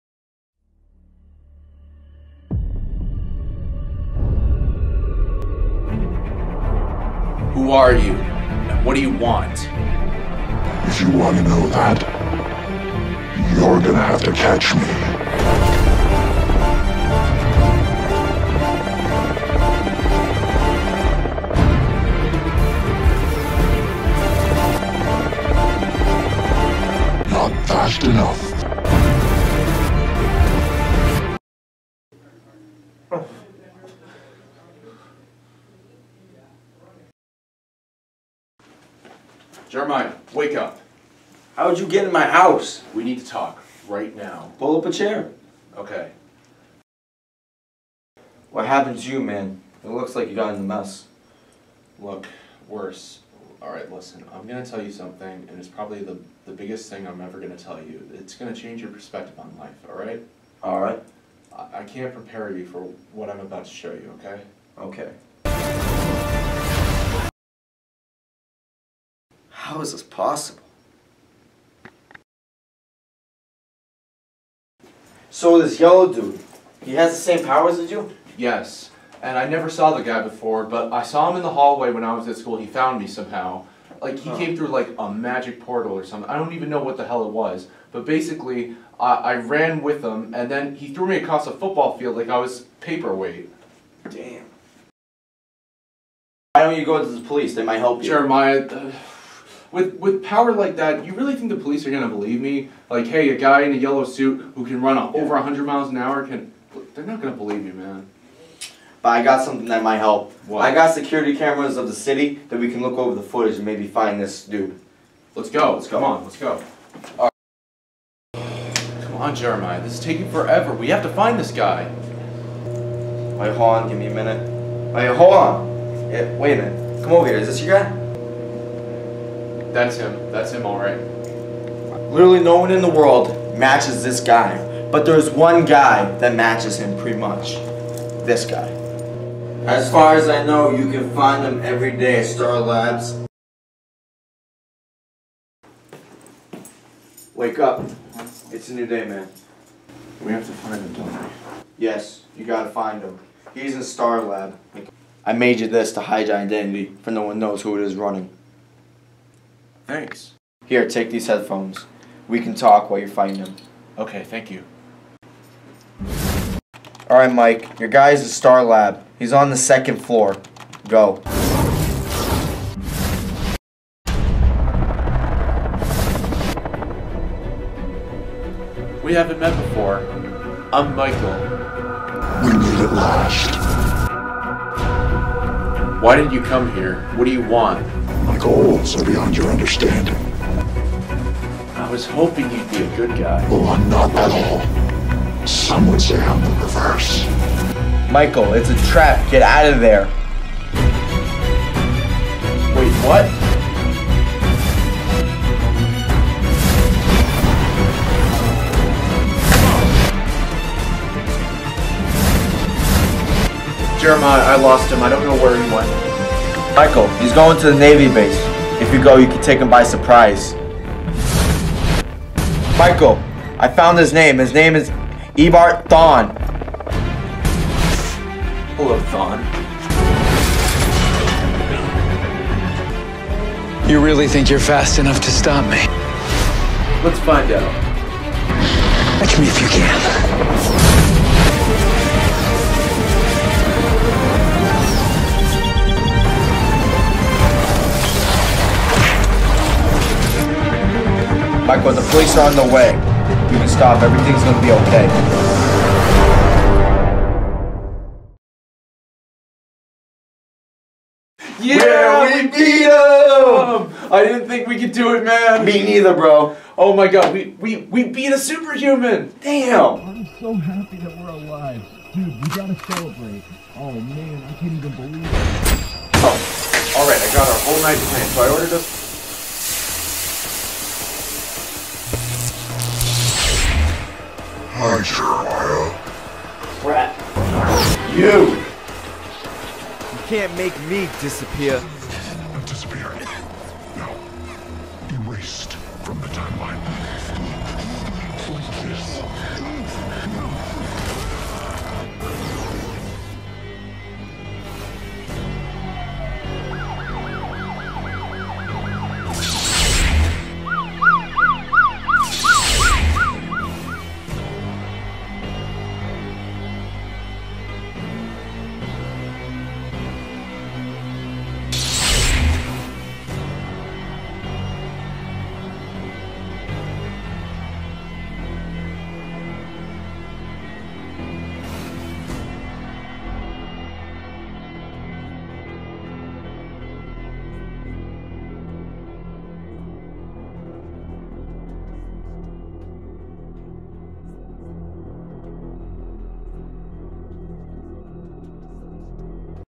Mike As The Flash part 2: Green screen ￼￼2018
This was a film I had made in 2017 back in high school. I had learned how to edit through making this short film. Without a doubt I am a big fan of The Flash. So I made a spin-off and green-screened myself in.

In no way do I owe the rights to these characters. This is a fan made short and a remake.